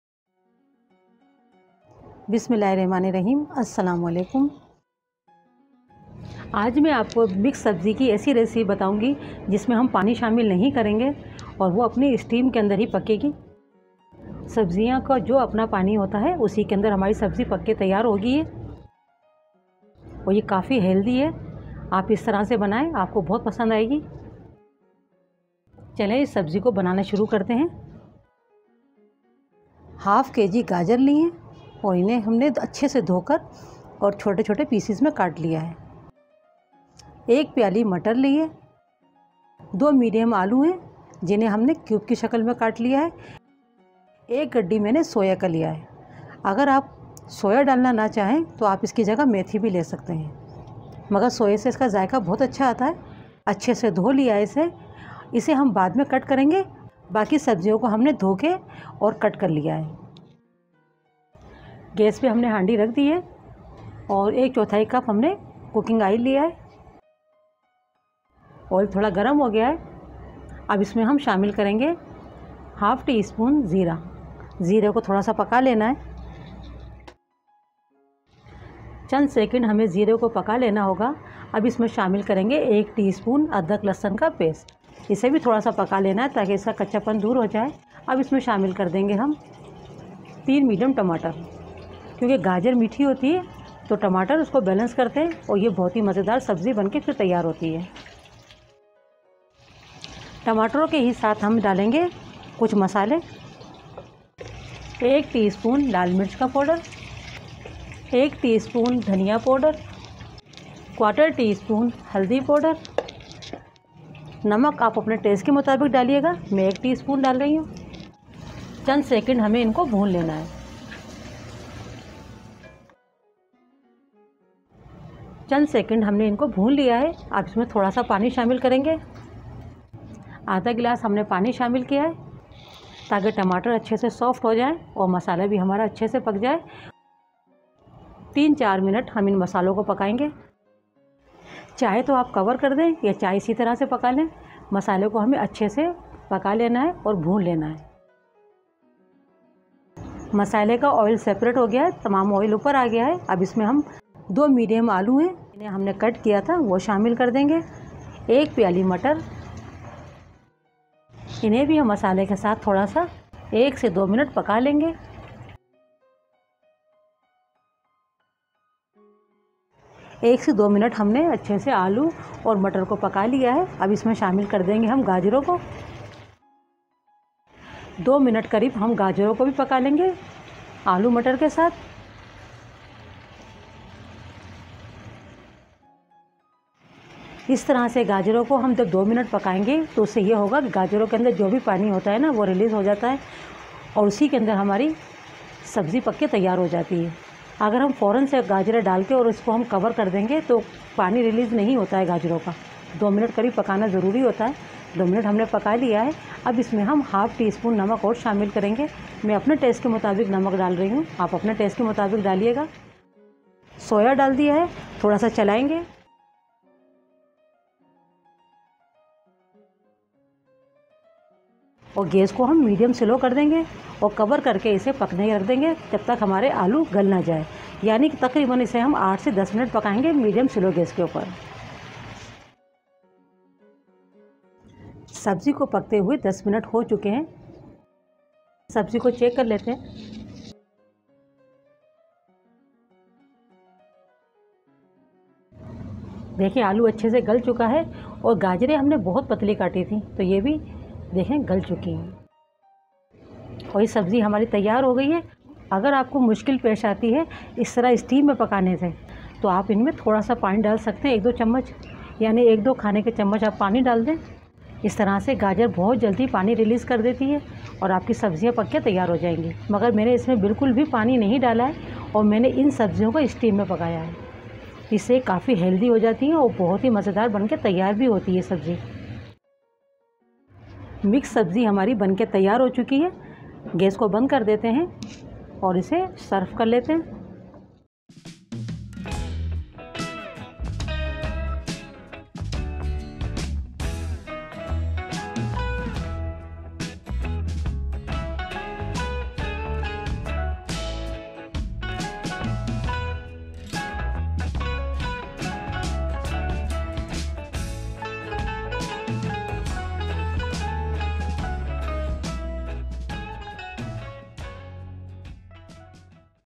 Aloo matar gajar ki mazedar sabzi | Mix vegetable recipe | mix veg sabzi
I love this recipe because it’s easy, quick and can be made with any type of vegetable. I’ve made this recipe with fresh vegetables, but this recipe can be made with a pack of frozen vegetables of your choice.

mix vegetable recipe indian. Mix veg recipe dry. mix vegetable recipe Pakistani. kadai veg recipe restaurant style. mix veg sabzi